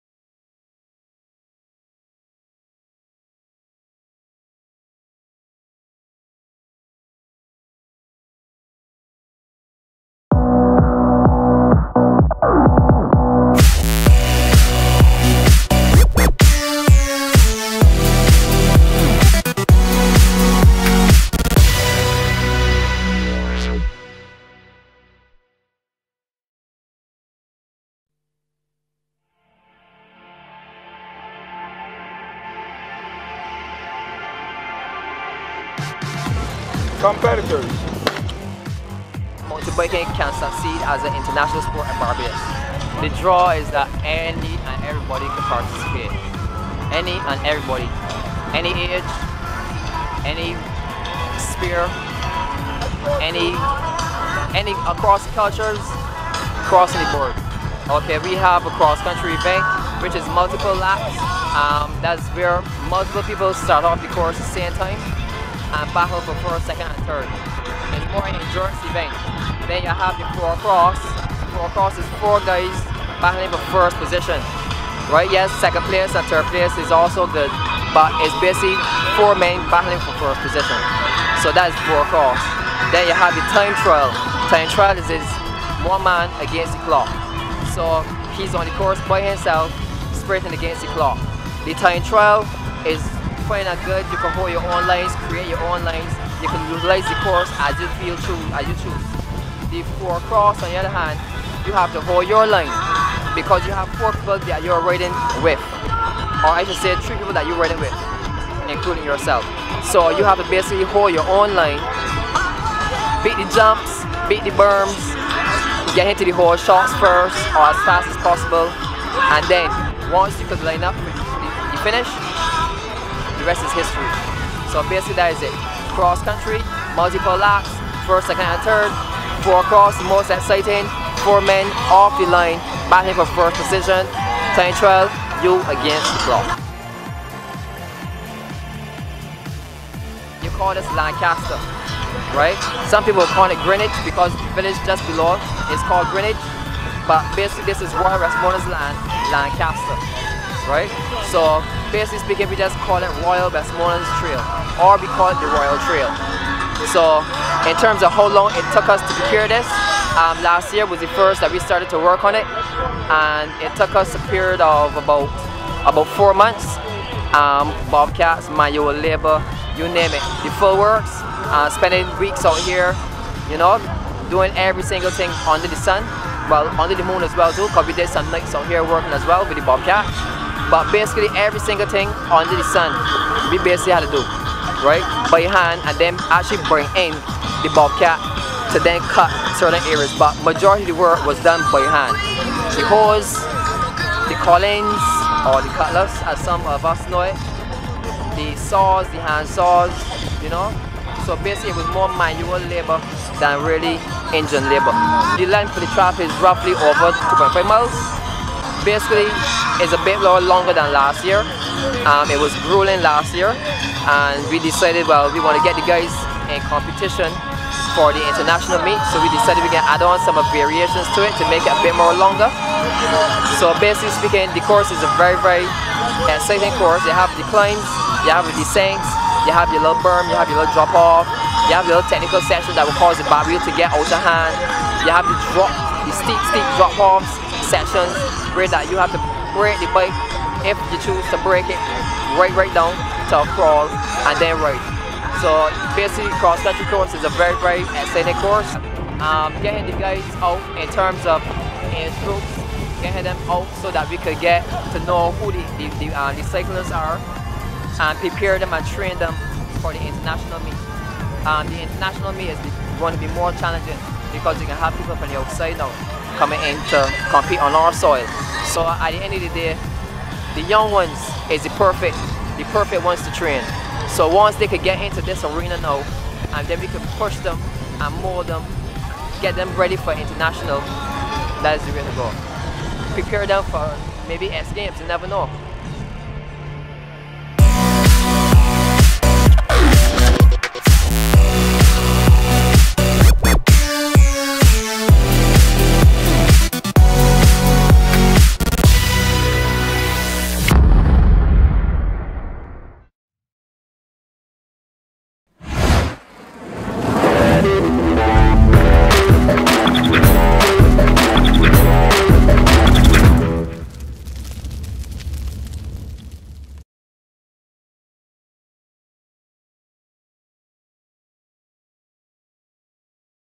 Adrenaline Pulse - Mountain Bike 4XFour Cross Event
Adrenaline Sun visited the 'Royal Trail Downhill Mountain Bike Event' at Lancaster in Barbados in this webisode of 'Adrenaline Pulse'.